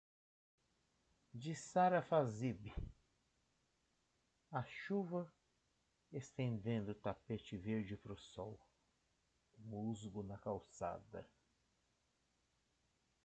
740 - A chuva - Sara Fazib
Um haicai da escritora brasileira Sara Fazib:

a chuva estendendo
tapete verde pro sol
musgo na calçada
(Sara Fazib)